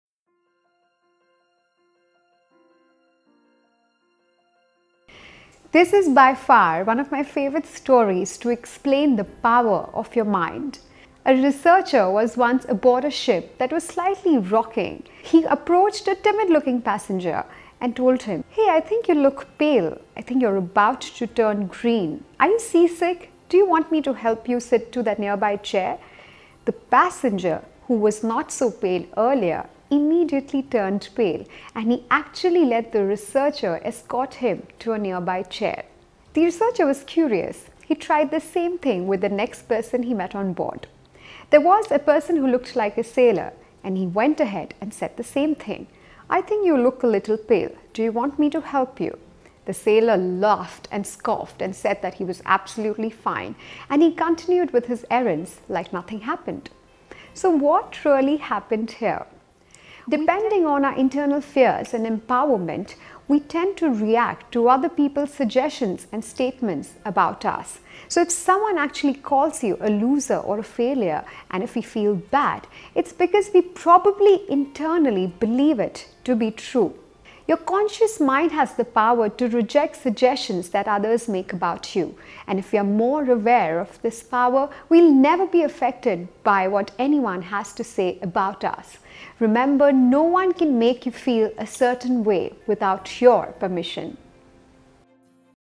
No One Can Put You Down - Secrets of A Powerful Mind
The human mind is full of many fascinating secrets. Do you feel bad if someone calls you a 'failure'? If yes, do you know why? Listen to find out!

Dr. Shamoly Khera is a Confidence Expert & Communications coach. She is the Founder of Speak To Inspire Coaching.